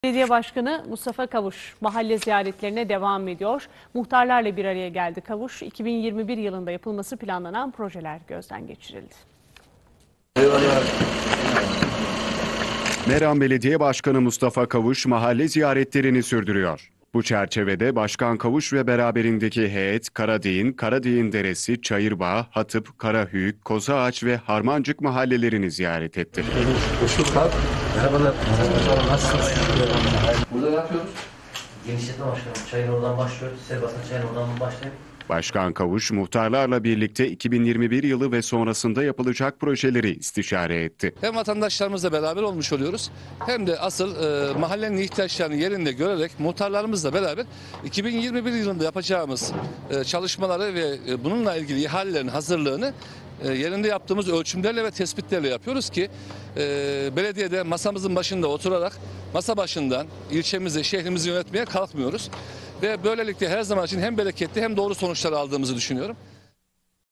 MERAM DA MAHALLE ZİYARETLERİ SÜRÜYOR
Meram Belediye Başkanı Mustafa Kavuş, mahalle ziyaretlerine hız kesmeden devam ediyor. Bu çerçevede Meram’ın 7 mahallesinde daha incelemelerde bulunan Başkan Mustafa Kavuş, mahalle muhtarlarıyla da 2021 ve sonrası için yapılacak çalışmaların planlamasını da gerçekleştirdi. Başkan Kavuş, yaptığı bu mahalle ziyaretlerinin Meram’ı masa başından değil yerinden yönettiklerinin bir göstergesi olduğunu söyledi.